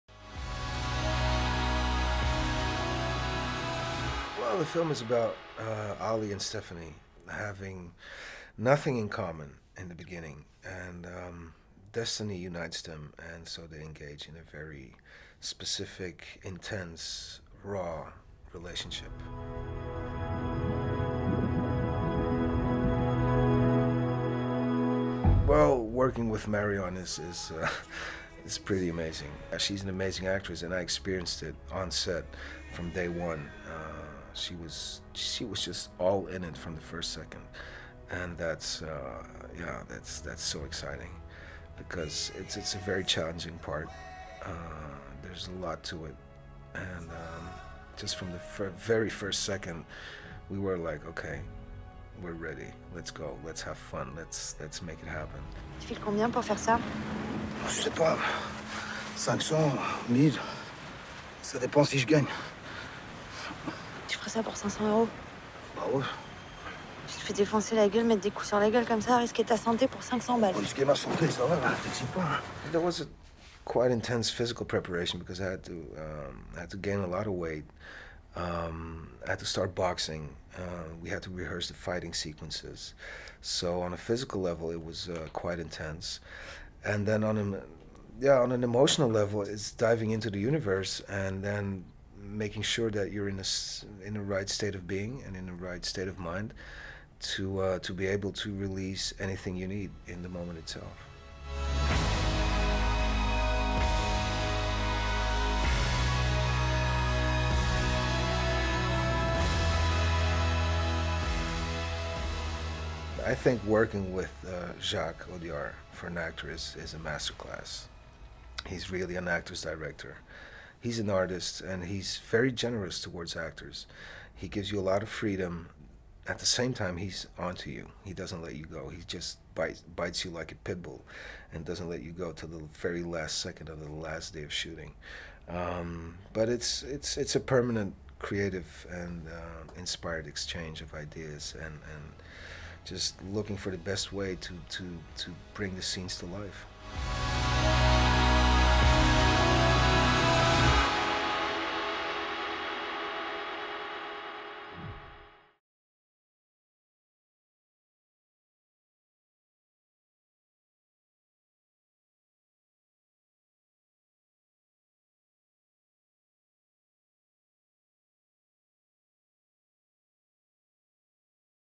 SOHK.TV interviews Matthias Schoenaerts (Rust and Bone)
SOHK.TV interviews Matthias Schoenaerts (Bullhead) star of Jacques Audiard's latest film Rust and Bone (De rouille et d'os).

Schoenaerts discusses how he prepared for his physically demanding role as a street fighter, the emotional heart of the film's story, and what it was like to work with Marion Cotillard and one of cinema's greatest directors, Jacques Audiard.

Filmed at the 56th BFI London Film Festival, October 2012